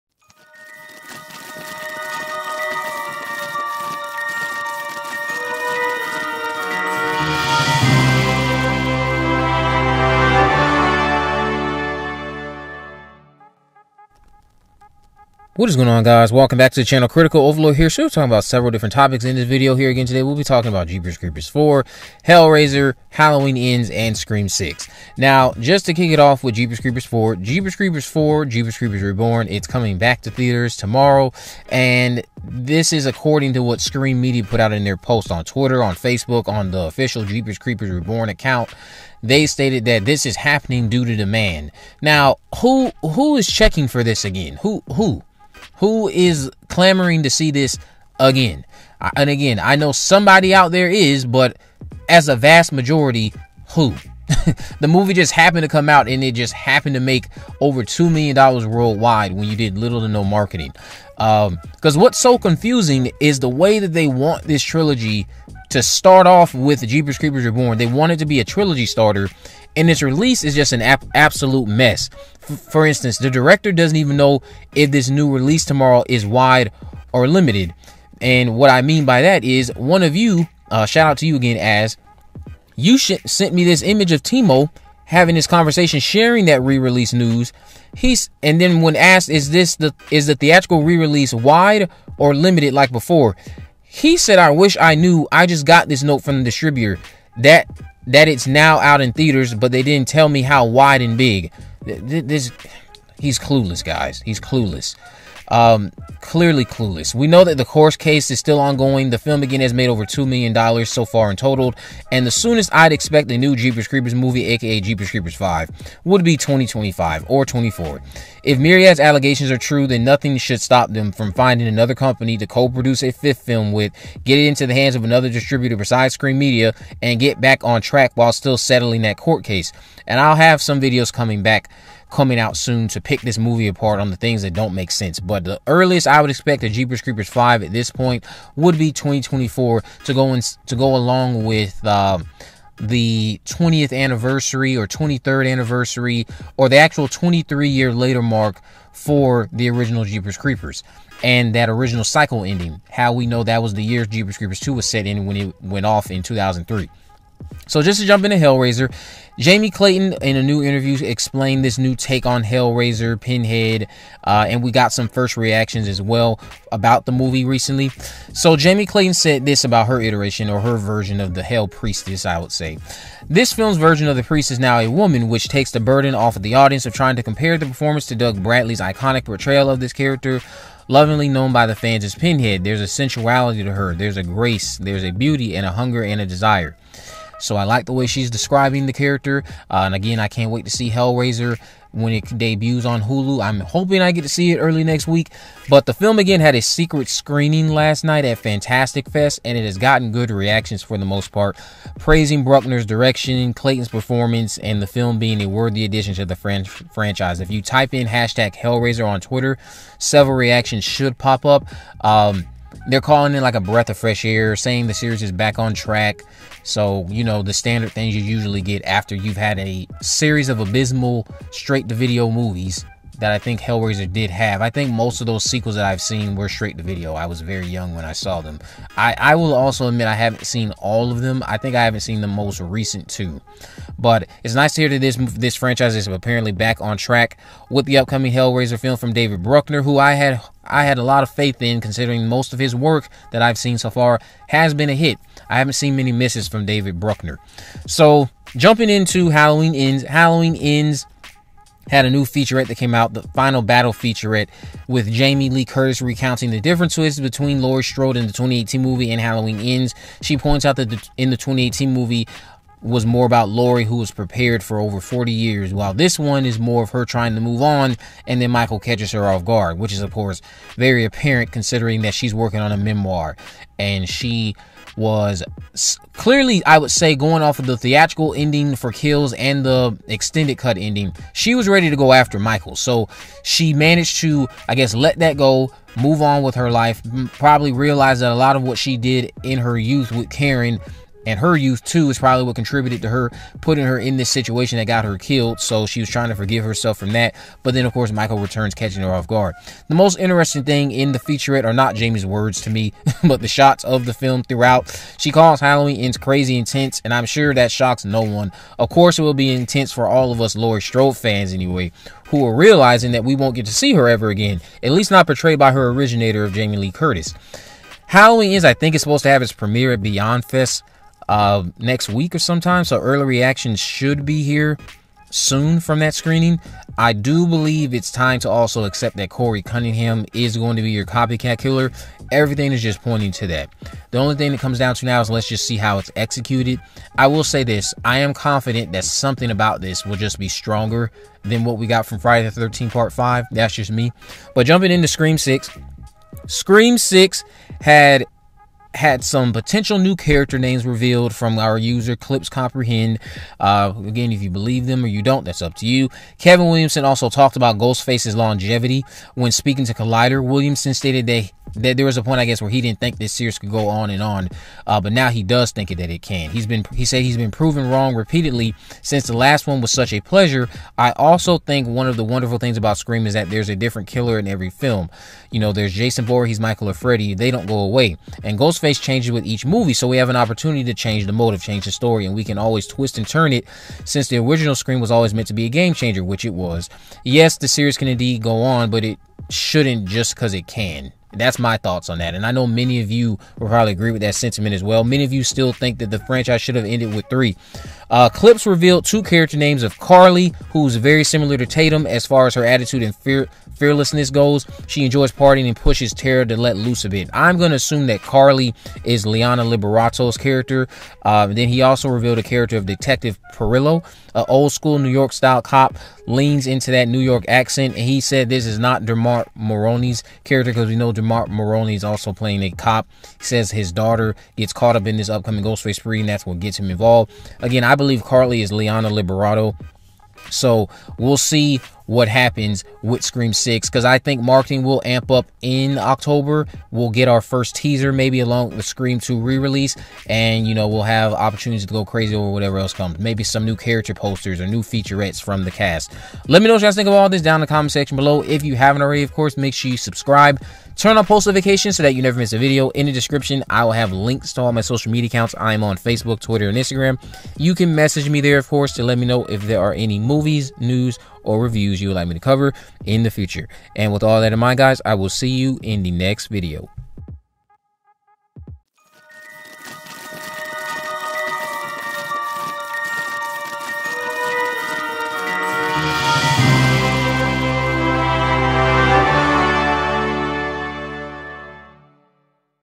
Jeepers Creepers 4 NEW Release, Scream 6 Update, Hellraiser Reactions, Halloween Ends Update
Jeepers Creepers 4 is returning to theaters this Friday unfortunately due to "demand". Scream 6 character names continue to be revealed via the insider clips_comprehend. Halloween Ends new featurette teases the final battle between Michael Myers and Laurie Strode. David Bruckner's Hellraiser premiered at Fantastic Fest last night to positive feedback overall.

0:00 Jeepers Creepers 4
2:37 Hellraiser
4:09 Halloween Ends
5:44 Scream 6

Art By: Steven Luros